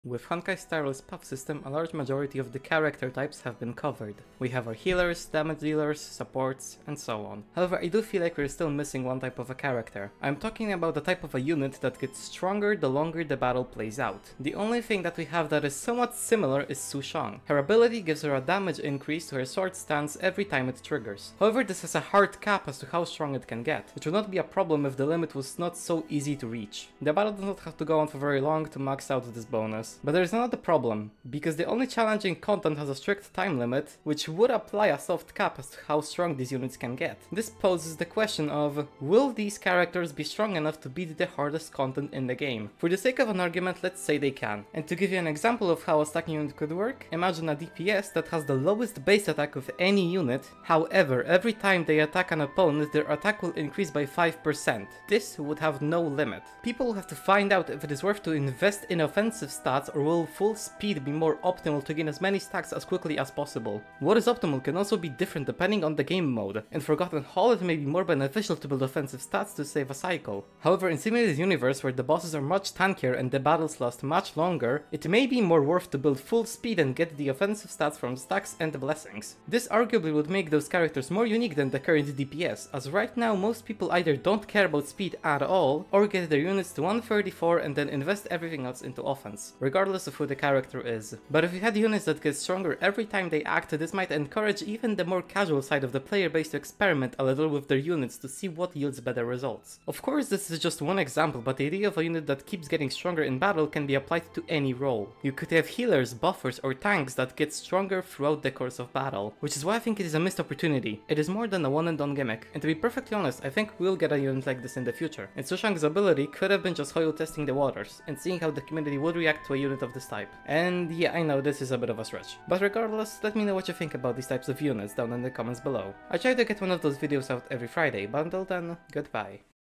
The missing character archetype in Honkai Star Rail | Game Talks
Bare in mind that all that this series is simply me, looking at different aspects of games I cover and throwing out ideas, to see what others think.

Do you think there are any other character archetypes that are missing from HSR, be sure to let me know.